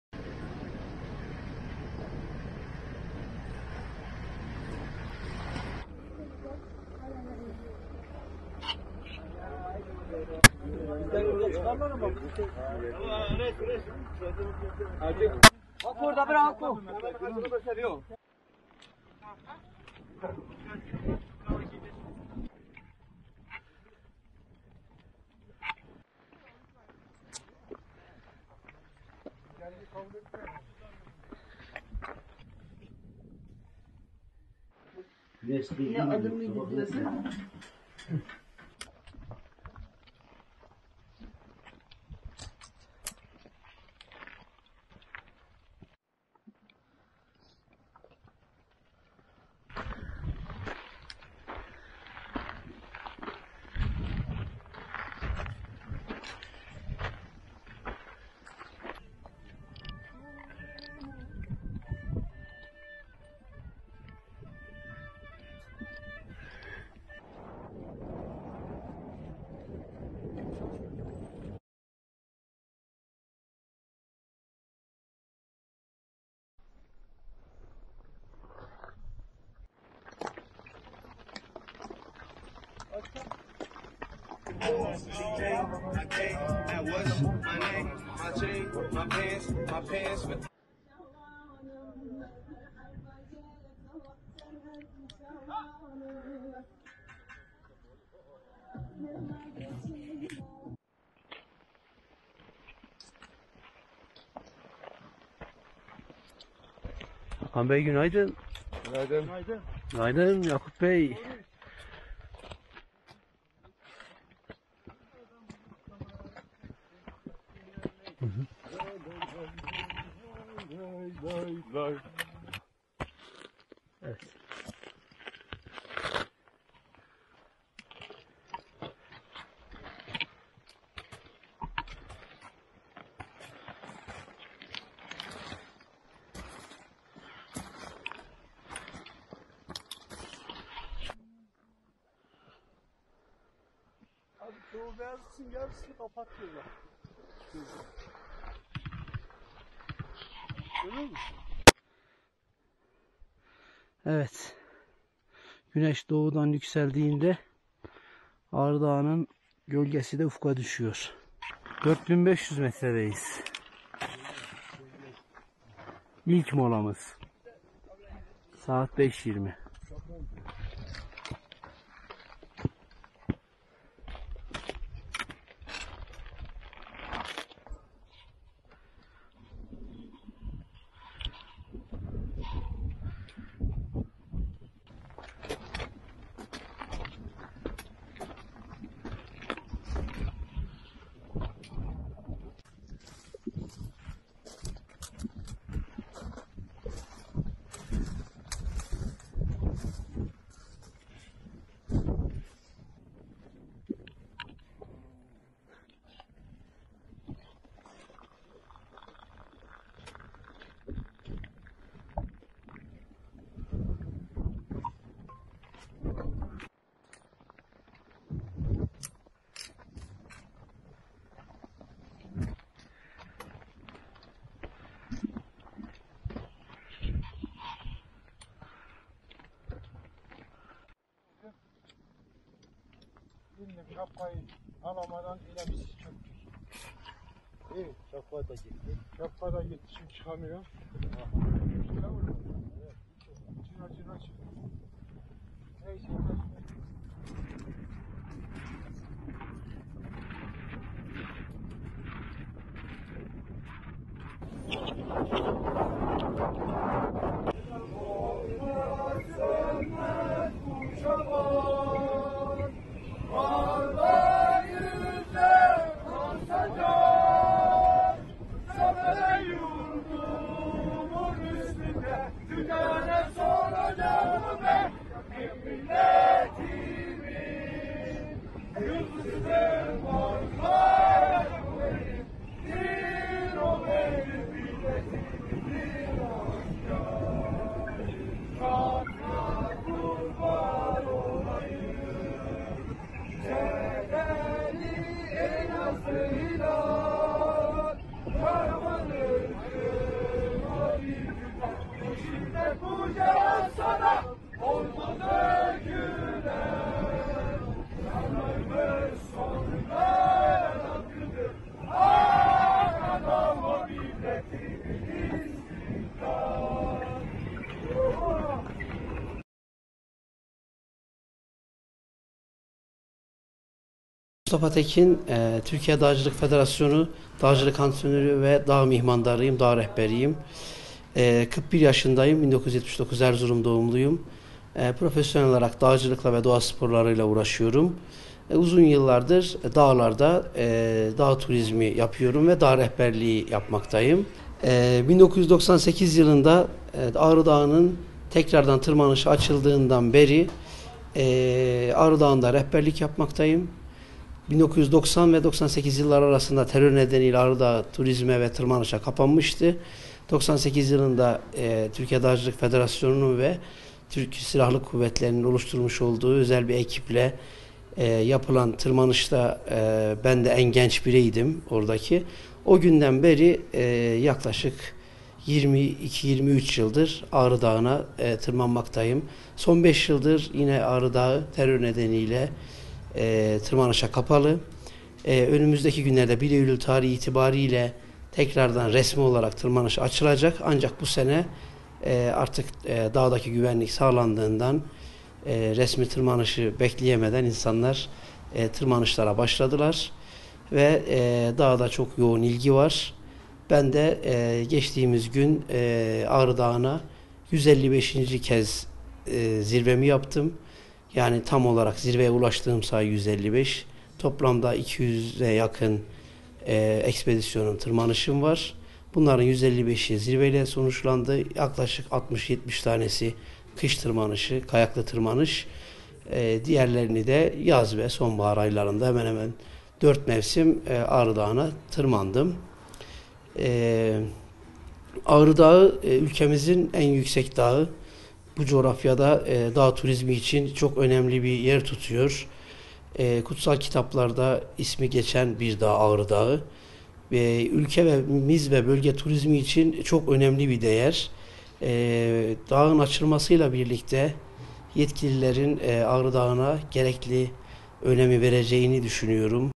Ağrı Dağı'nın zirvesini tam 155 kez gördü
◥ Alpin Doğa Sporları Kulübü Başkanı ve aynı zamanda dağ kayağı ile tur rehberliği yapan 41 yaşındaki Mustafa Tekin, Türkiye'nin çatısı olarak adlandırılan Ağrı Dağı'na 155'inci kez zirve tırmanışını gerçekleştirdi. Hayatının büyük bir bölümünü evinden çok dağlarda geçiren Tekin, "Zirveye her ulaştığımda ayrı bir zevk yaşıyorum" dedi.
◥ 29 yaşında Atatürk Üniversitesi Erzurum Meslek Yüksekokulu, Turizm ve Otelcilik Bölümü son sınıf öğrencisi iken Ağrı Dağı'nın doruğuna 'kader arkadaşım' dediği Yıldırım Beyazıt Öztürk ile birlikte 50'inci kez tırmanışını o yıllarda 50'inci yılını kutlayan Atatürk Üniversitesi'ne hediye ettiğini ifade eden Tekin, geçen hafta yaptıkları tırmanışla bunu 155'e çıkardığını söyledi.
◥ "Evimden çok dağlarda yattım"